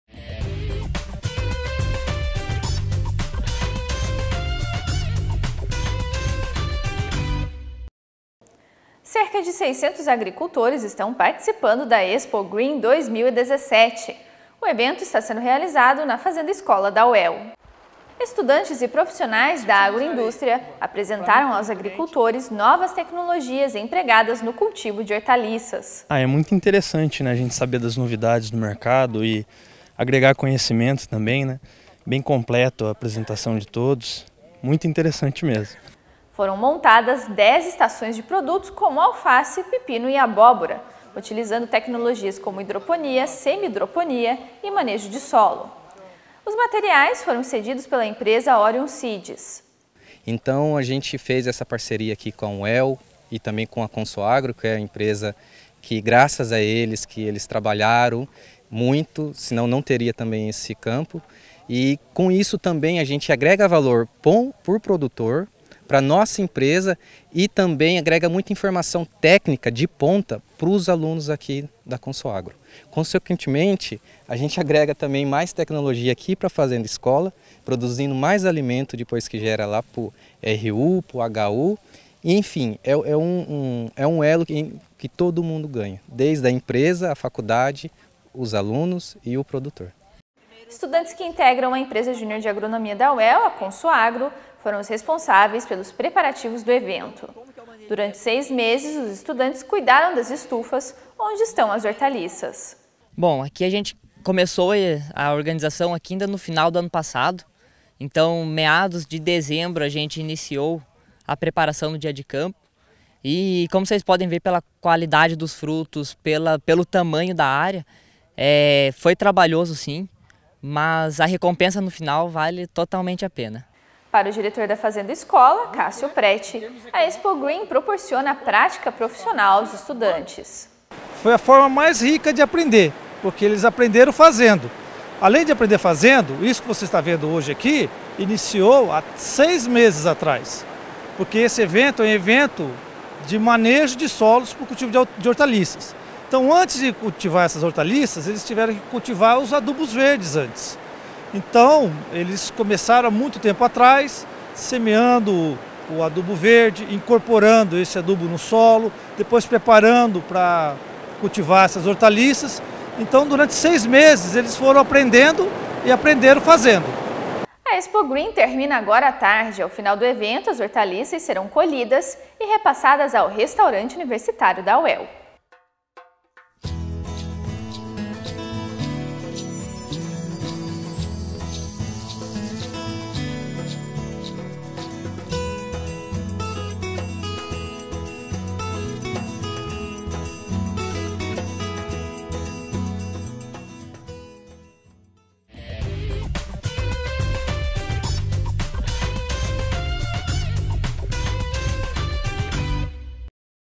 ExpoGreen mostra novas tecnologias de cultivo e manejo de hortaliças
Estudantes e profissionais da agroindústria apresentaram aos agricultores novas tecnologias empregadas no cultivo de hortaliças. A Expogreen termina agora a tarde. Ao final do evento, as hortaliças serão colhidas e repassadas ao Restaurante Universitário da UEL.